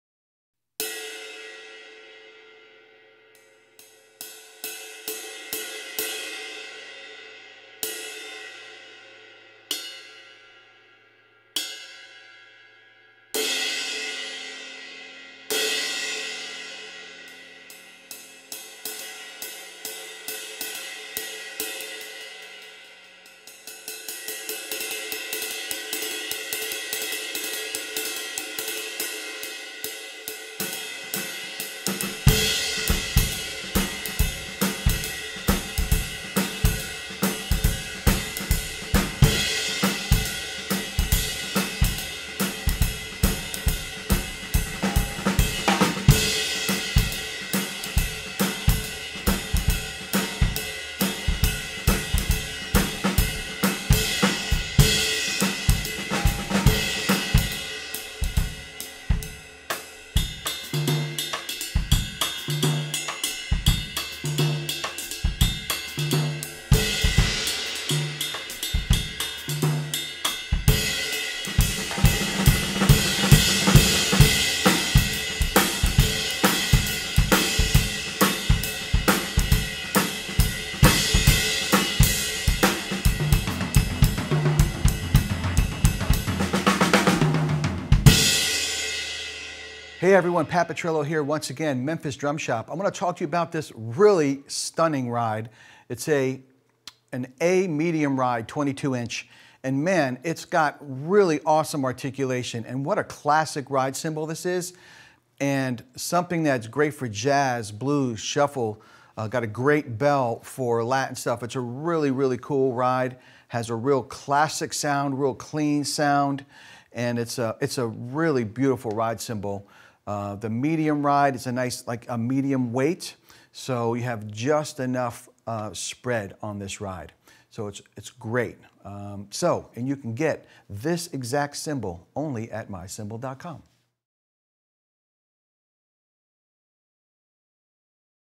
Zildjian 22" A Medium Ride Cymbal - played by Pat Petrillo - 2884g (A0036-1040623O)
Zildjian 22" A Medium Ride Cymbal available from myCymbal.com

Other gear used in this video:
• Cymbals (From drummer's L to R): Zildjian 13.25" K Custom Hybrid Hi-Hats, Zildjian 18" K Custom Special Dry Crash Cymbal
• Snare: Ludwig 14x6.5 Universal Walnut Snare Drum
• Drumheads: Remo Clear Powerstroke 3, Remo Coated Ambassadors
• Sticks: Vic Firth Extreme 55A